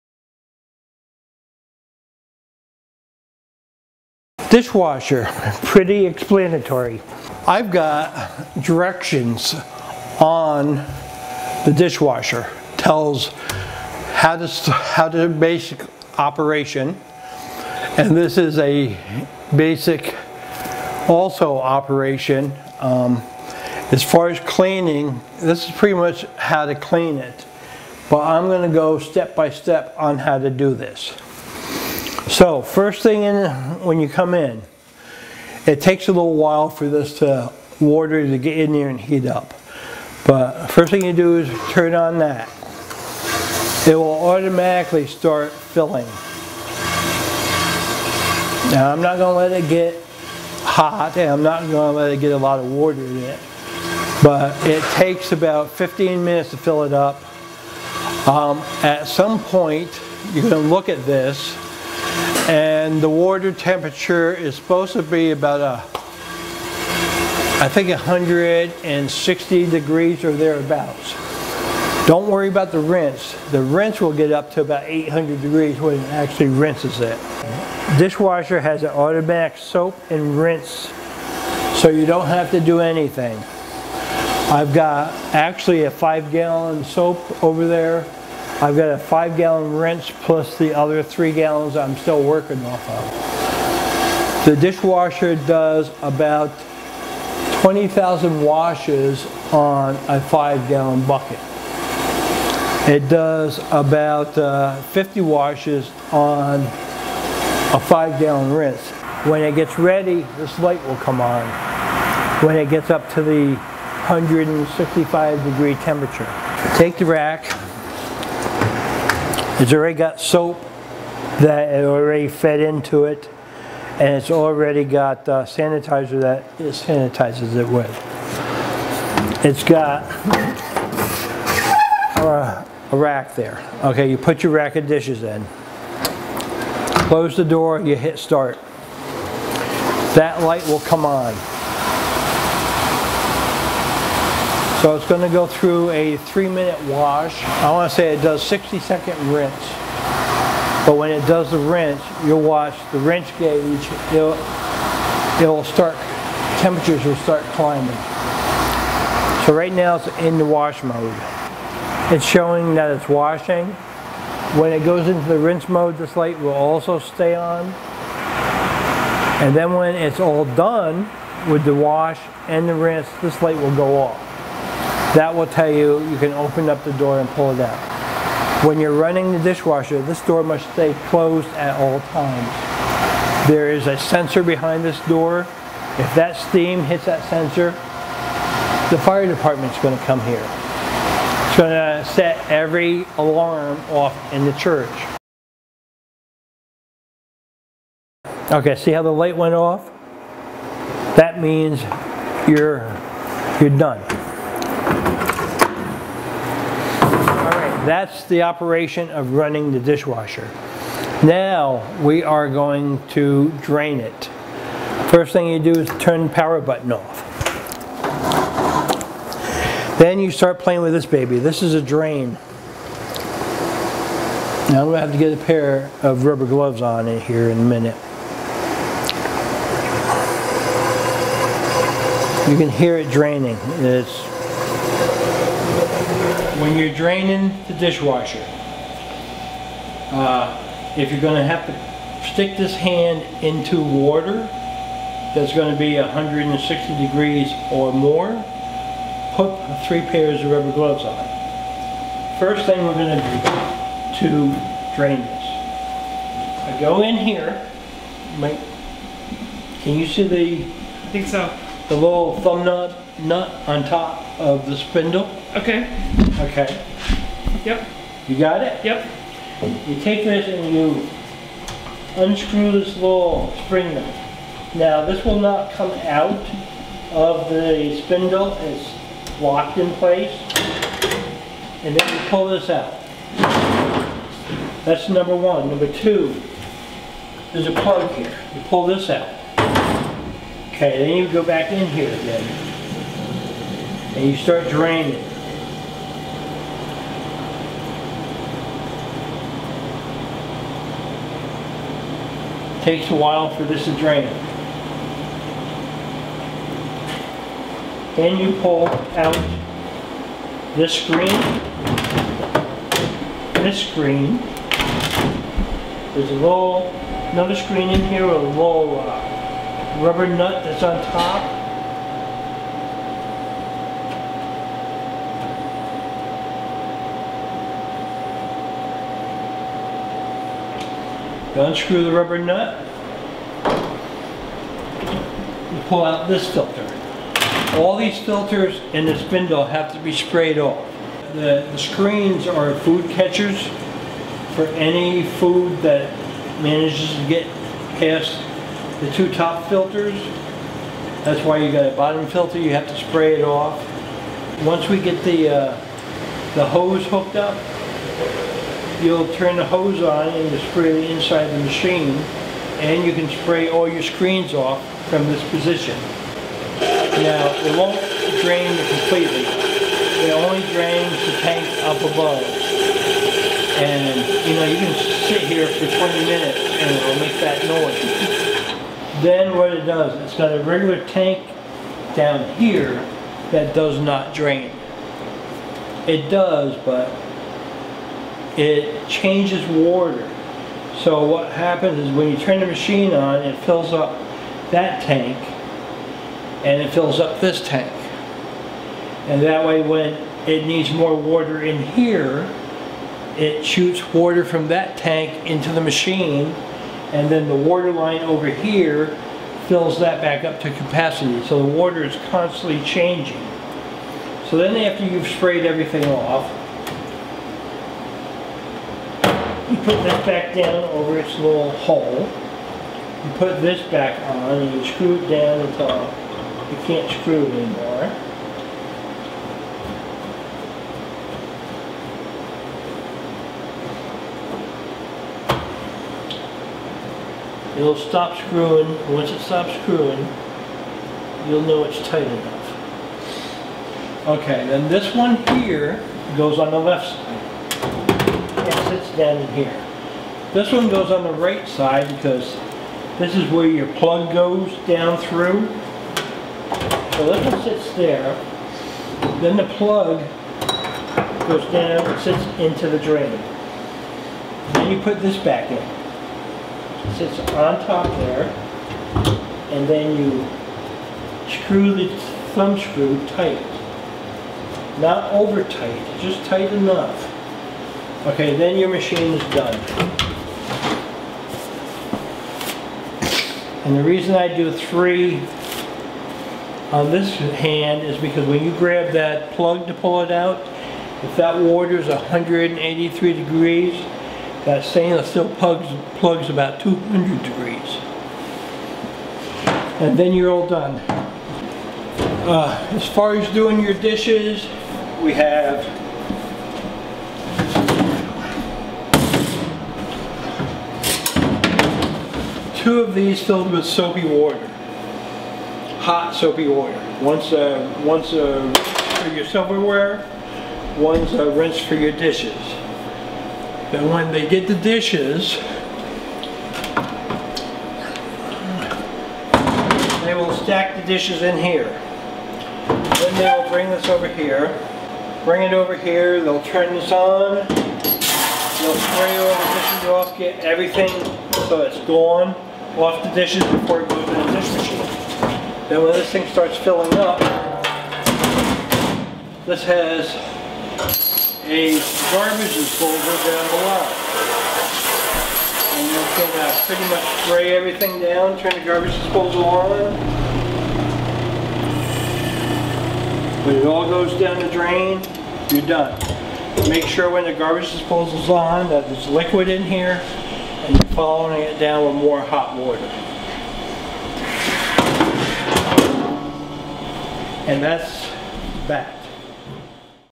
GSLC Kitchen: How to use the Dishwasher
Chef Rick teaches how to use the dishwasher.  Remember to keep the pantry door closed and manually drain it when finished.